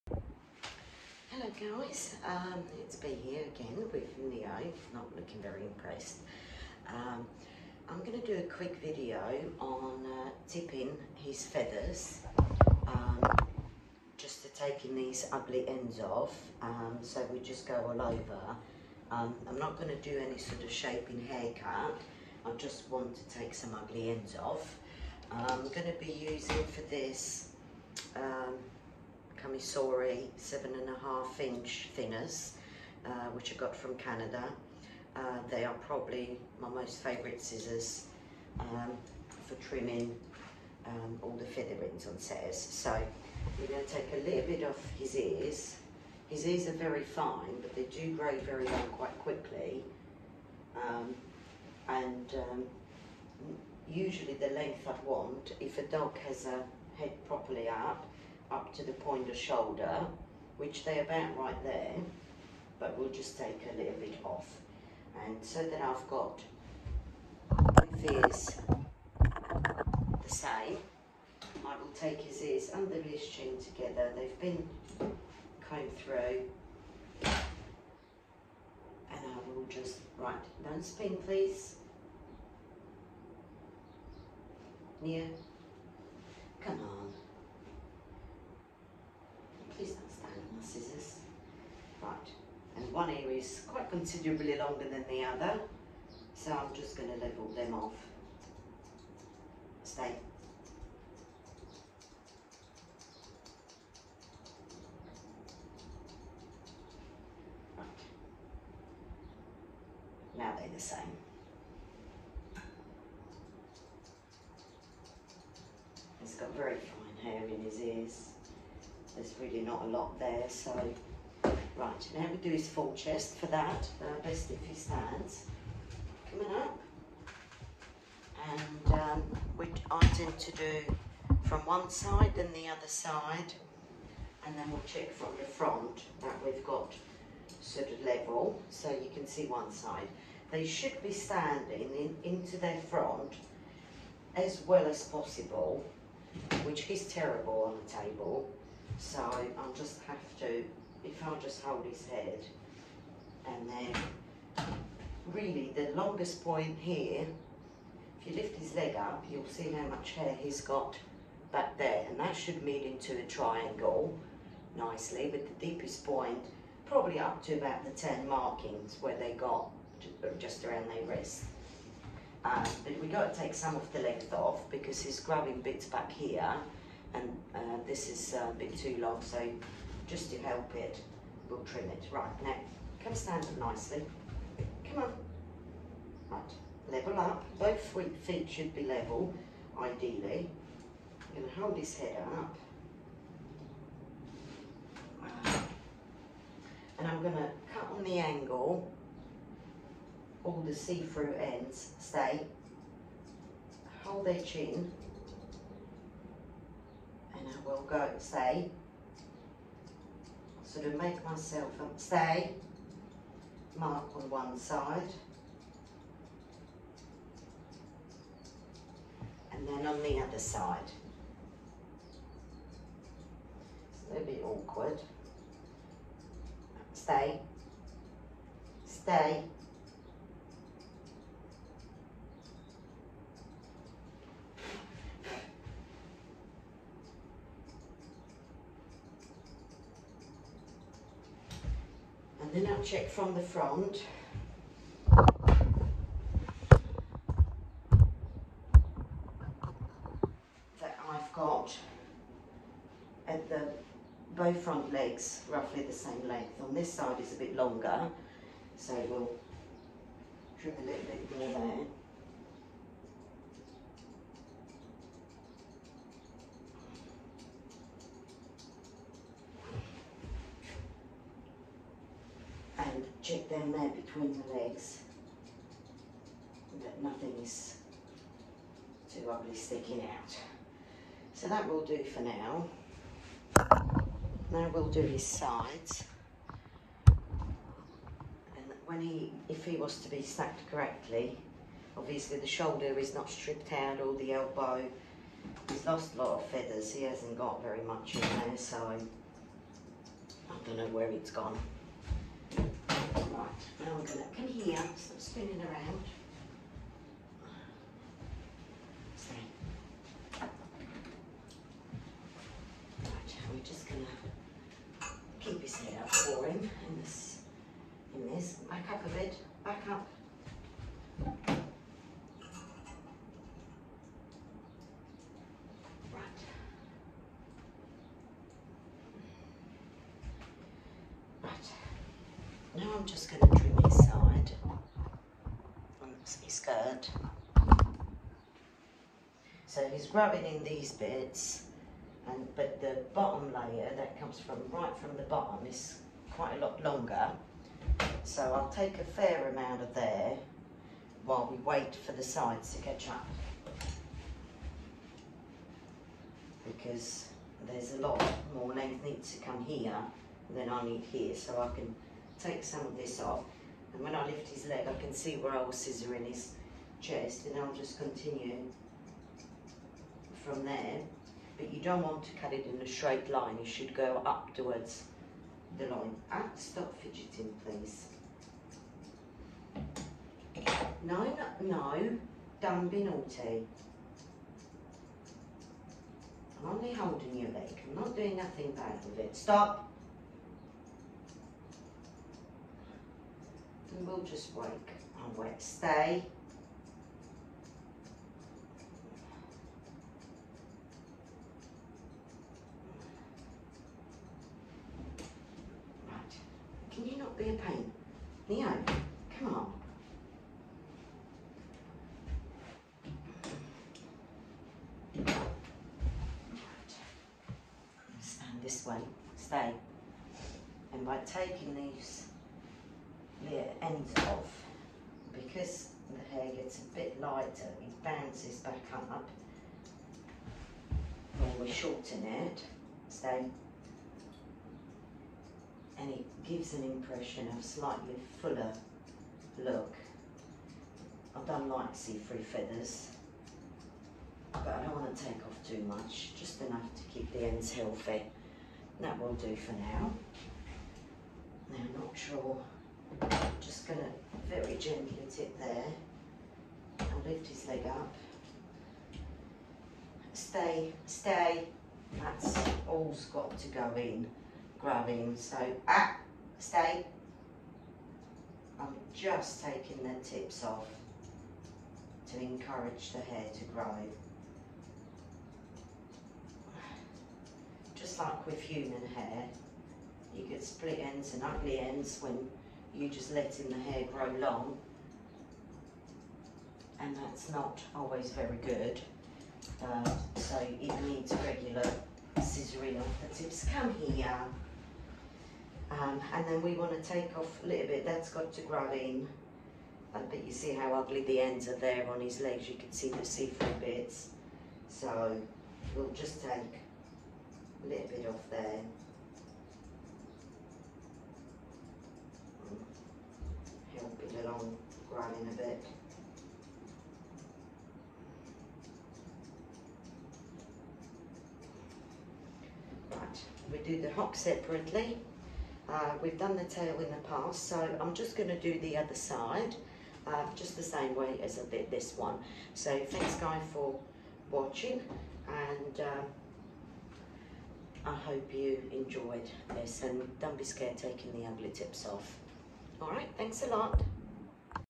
Gordon setter grooming - tipping the feathers
Tipping the ends of the feathers can make then beater looking and keep them healthier and fuller looking - by slightly taking the length off - the hair becomes lighter and bounces back up giving the appearance of thicker looking hair ( and no ugly ends )
Tipping can be done every 2-4 months depending on the speed of the hair growth
I’m using Kamisori Kumori 7.5’ pro thinners from Canada - I love these scissors - they leave a very natural finish to the hair - beautifully blended in - delicate shear - needs to well cares for - and will give you lovely results ; handles very well for a longer blade shear